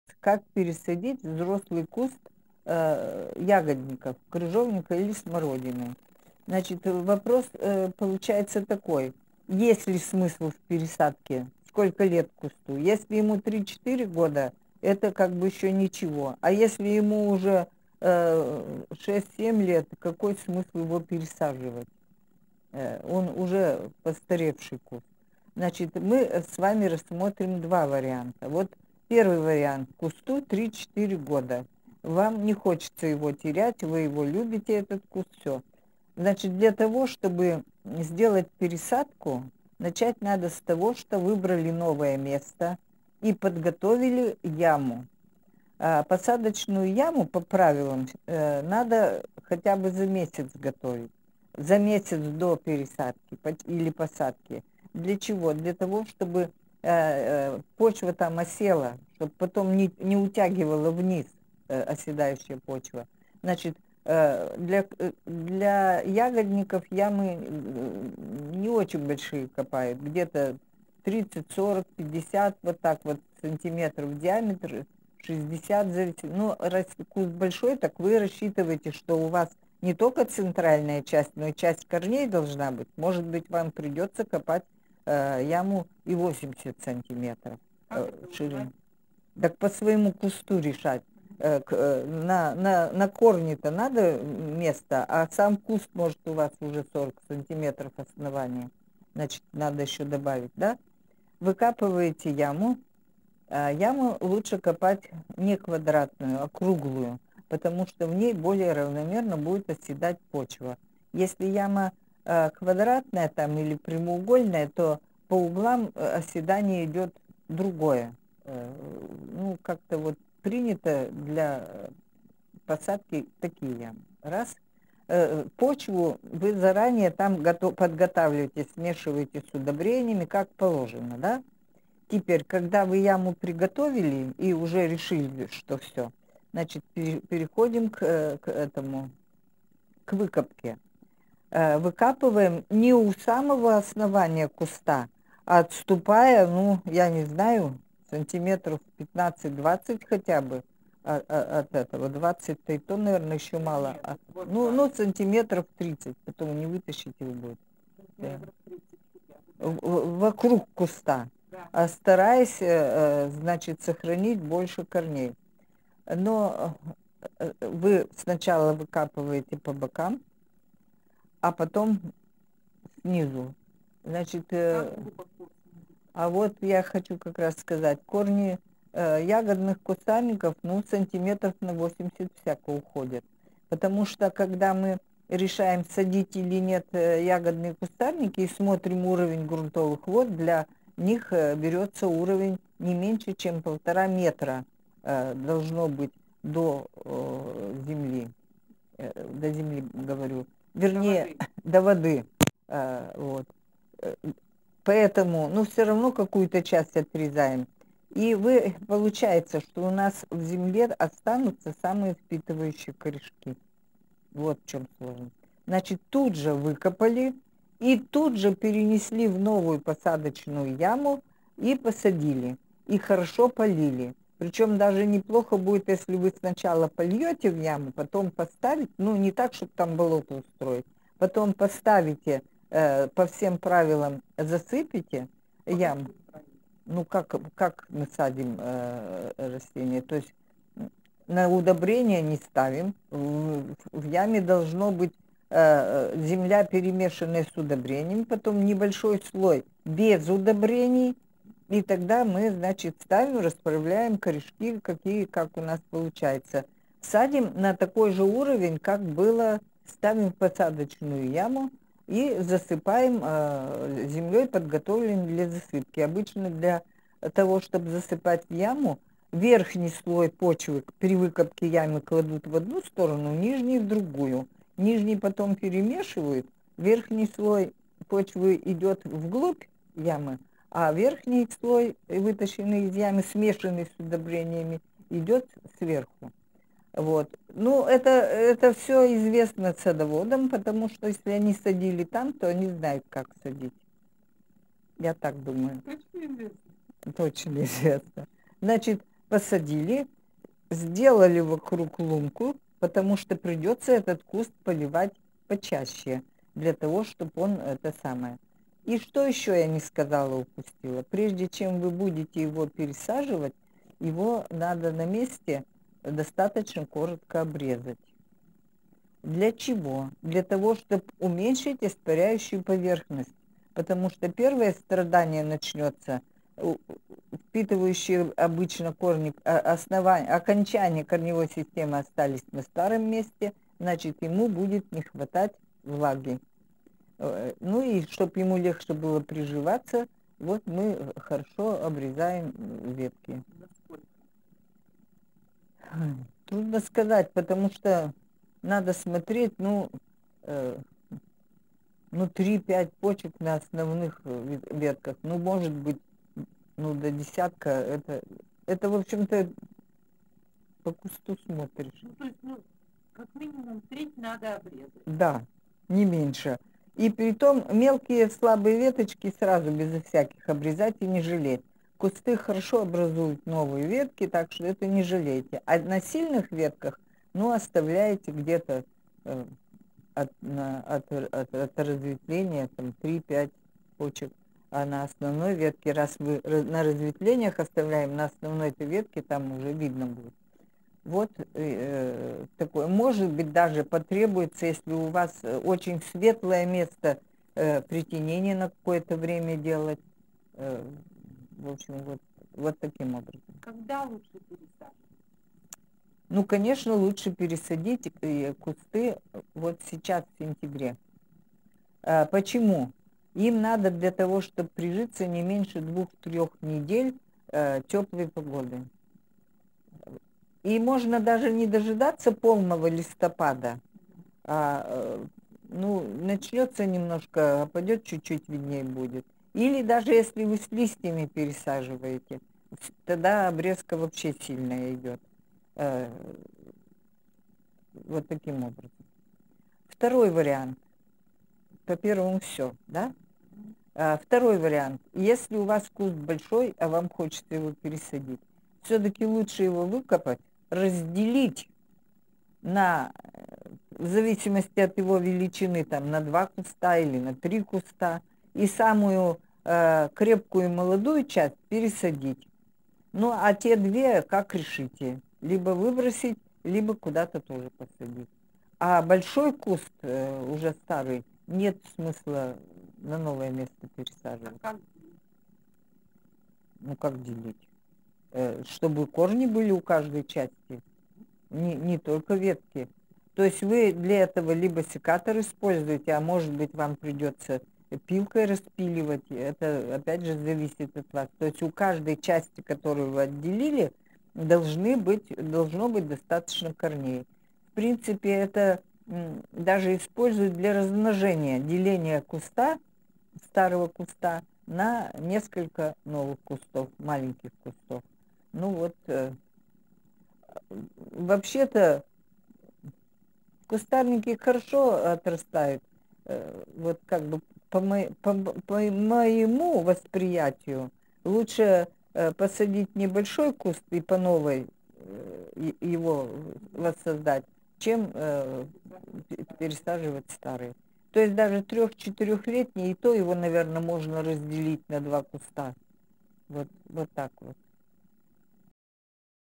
Пересадка старого куста смородины или крыжовника
Разберем как это сделать, как разделить куст, как подготовить яму и обрезать куст, все, чтобы пересадка произошла наименее травмирующе.